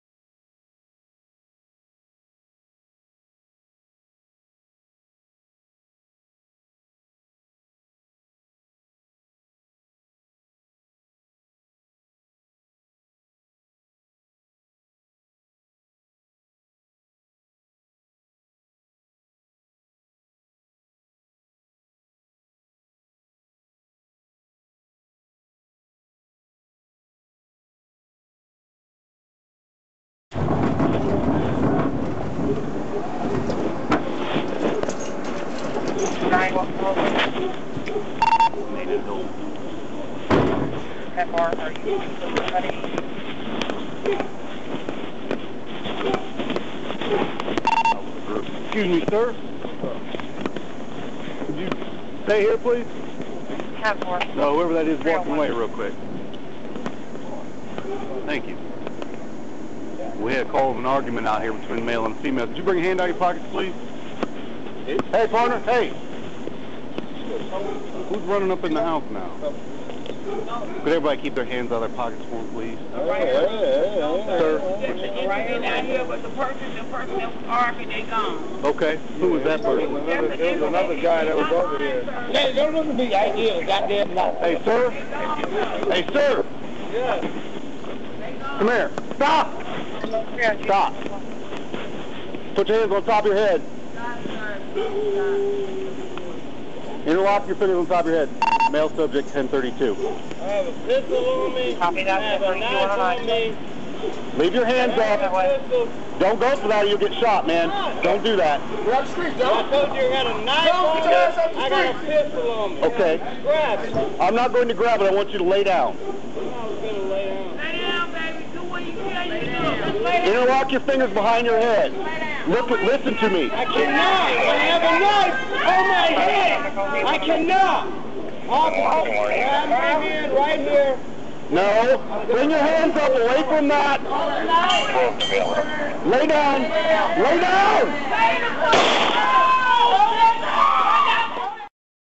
Dash Cam Footage from July 11 Officer Involved Shooting
This is dash cam footage from a Decatur Police squad vehicle of the July 11 officer-involved shooting in Decatur.  Video obtained from Decatur Police Department.